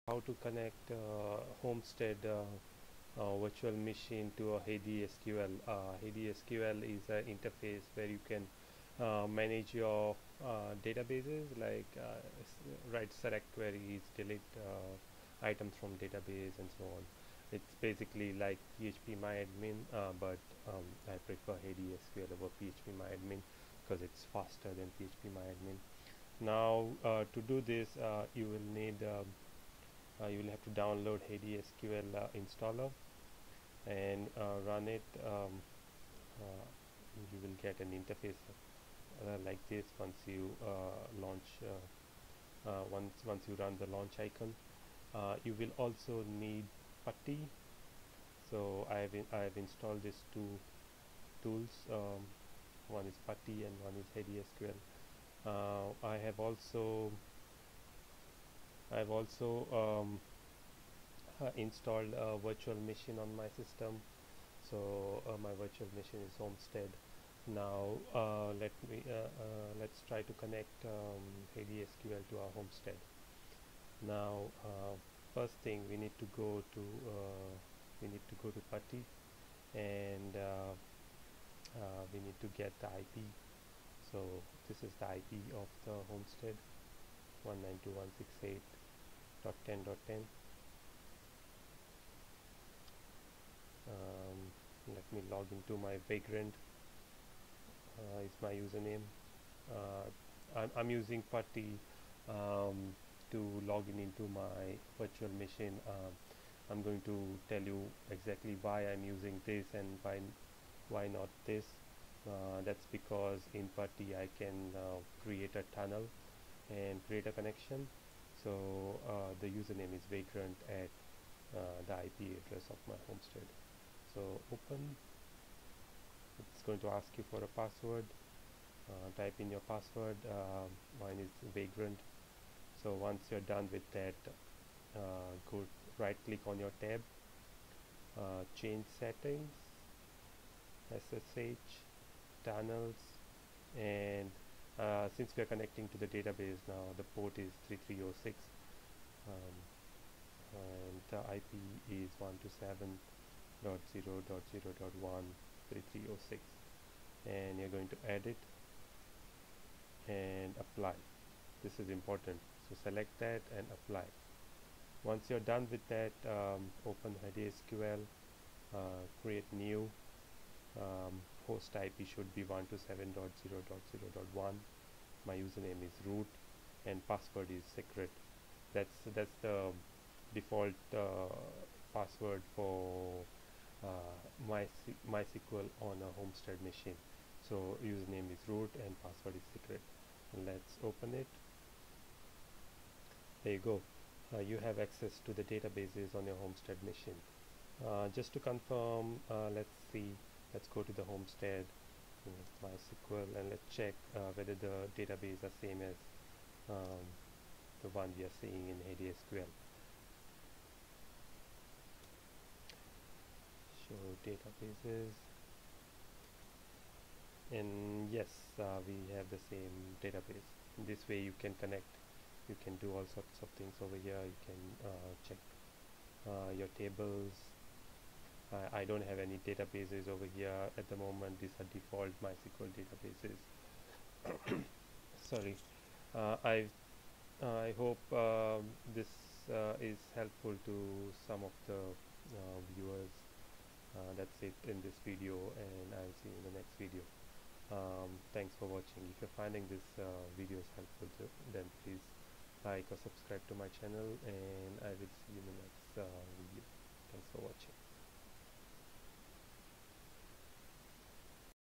Homestead VM tutorial - Access Homestead database from heidisql interface
In this video I am going to demonstrate how to access homestead virtual machine databases from Heidisql interface